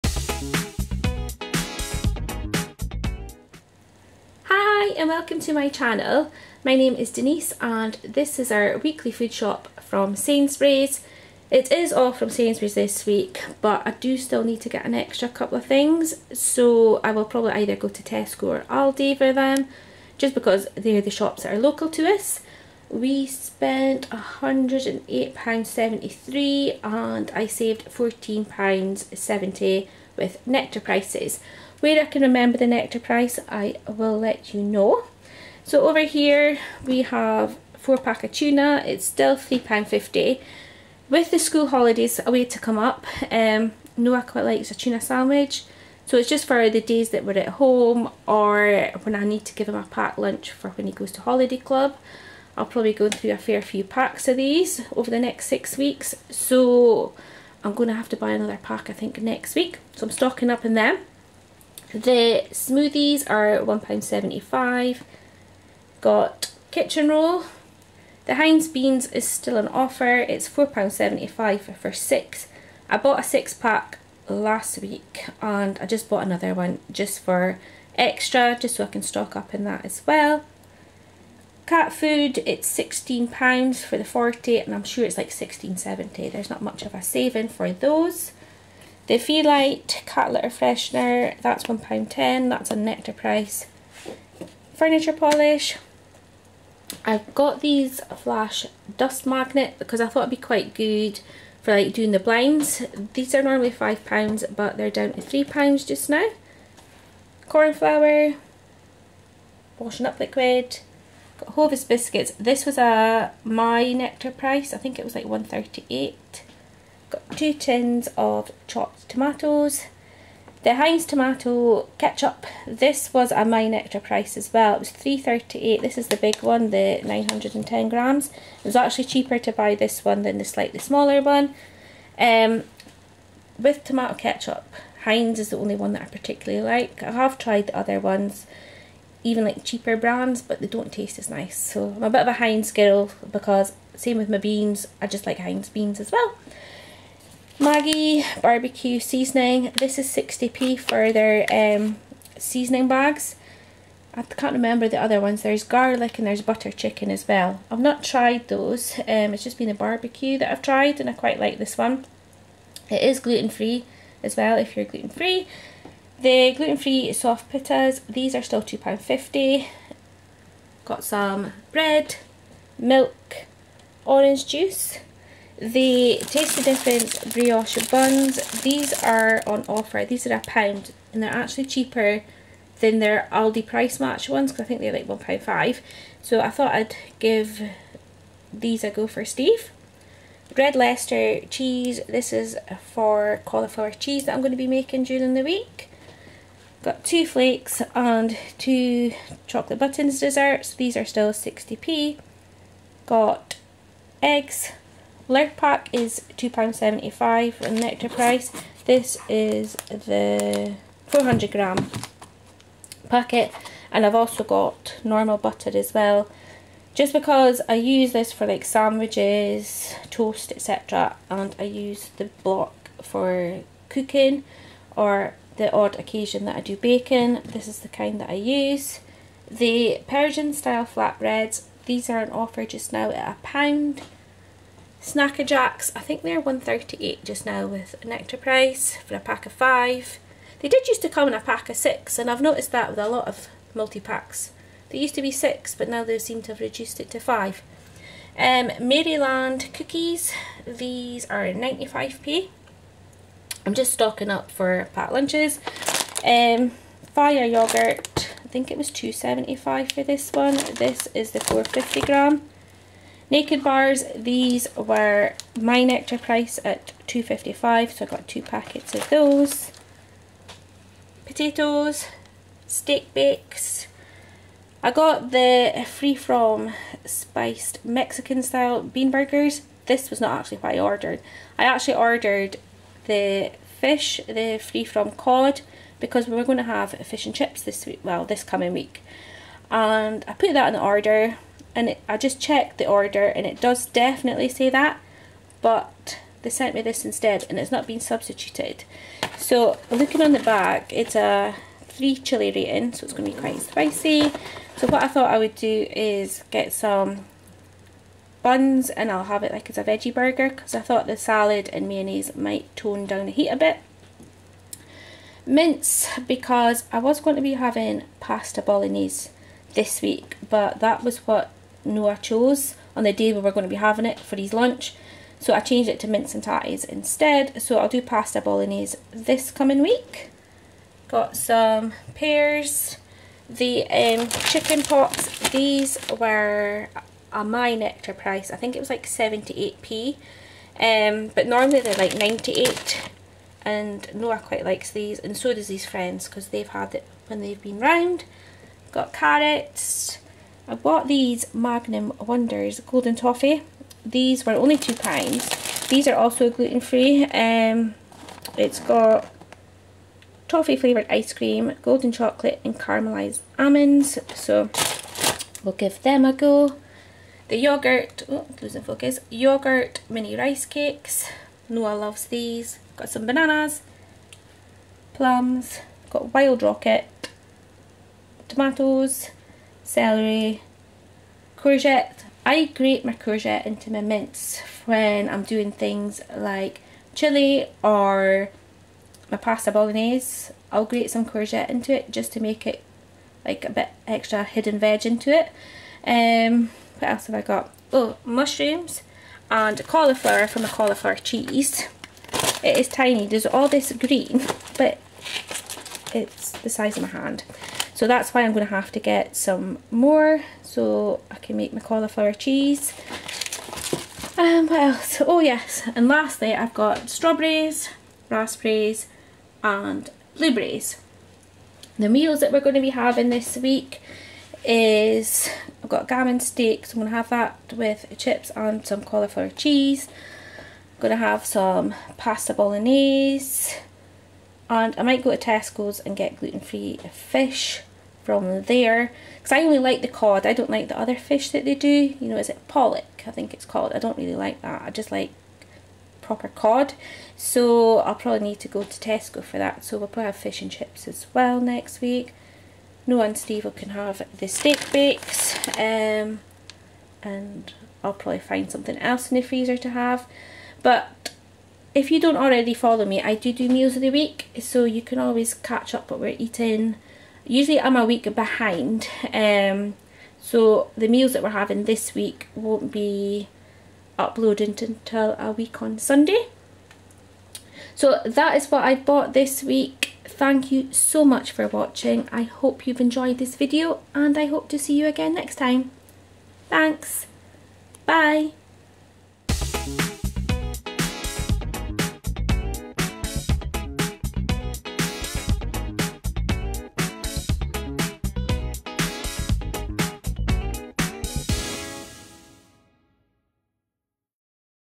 Sainsbury’s Grocery Haul | Weekly Food Shop | 28th June 2024
We are a family of three and this is our grocery haul from Sainsbury’s.  This food shop cost £108.73 and we saved £14.70 with Nectar prices.

✨This week’s meal plan includes:✨

Pasta Bolognaise
Gammon Steaks, Cauliflower Cheese & Chips
Fish & Chips

as well as a few other meals yet to be decided.